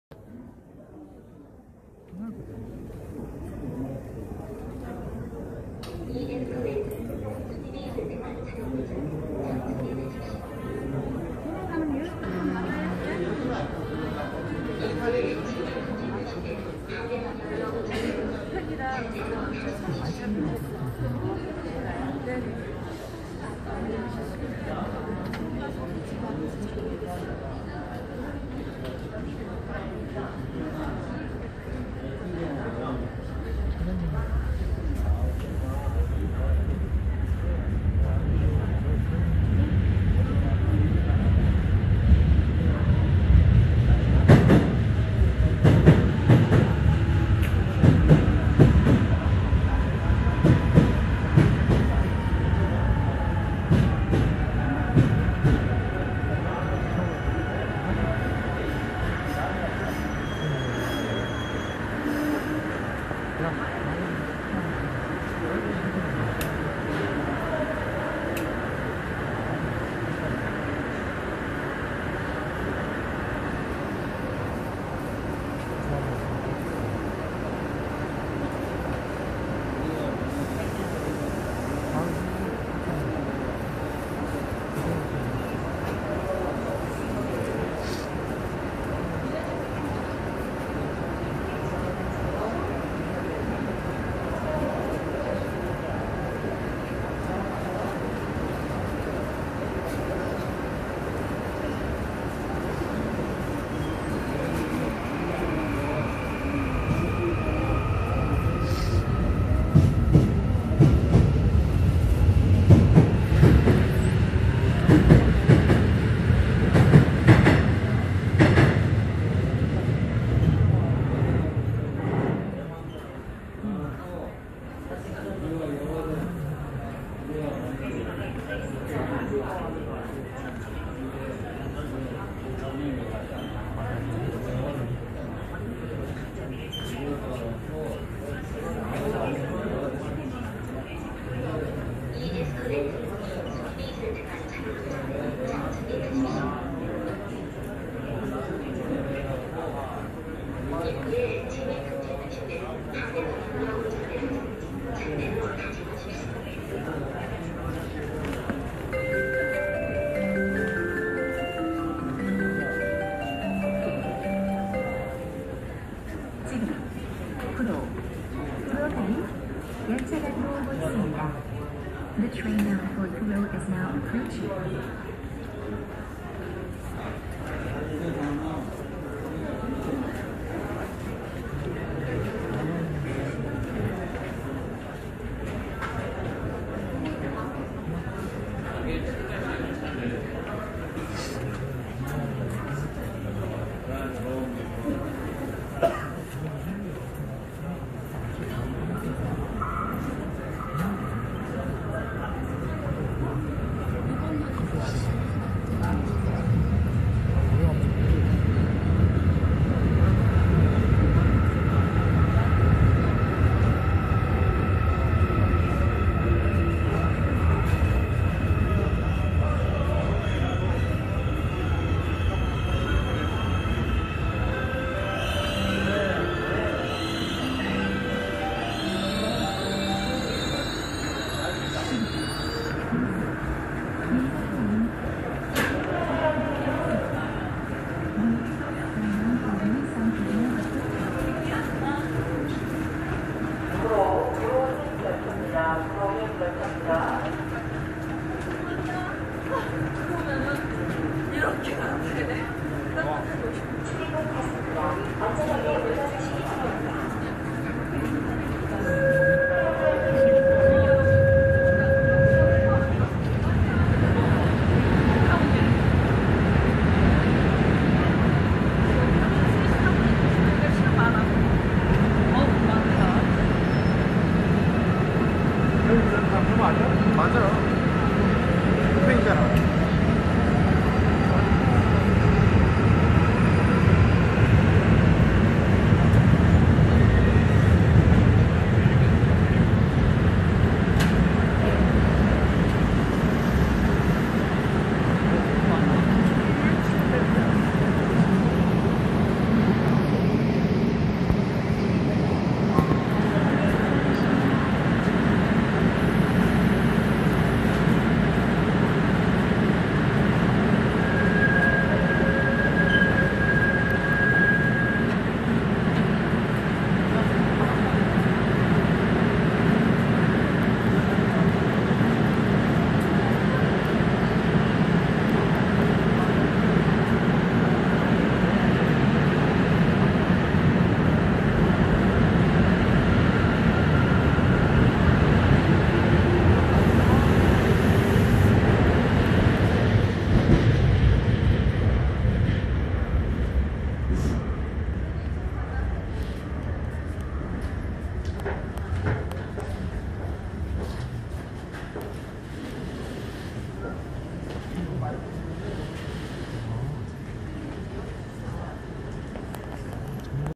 코레일 1호선 동인천급행 열차,용산급행 열차,구로행 열차 부천역 정,발차
東芝(도시바),日立(히타치 기반 우진산전)IGBT VVVF